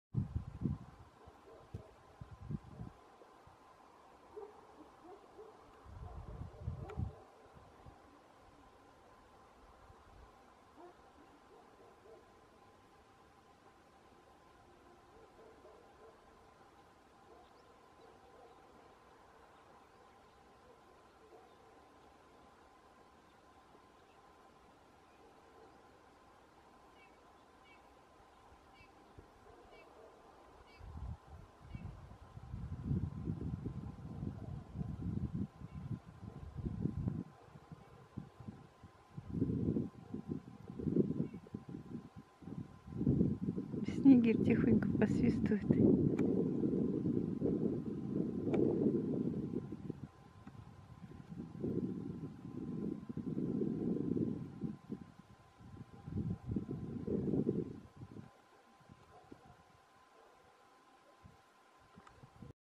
Снегирь 1 ноября
1 ноября шел снег,  а я заметила снегиря.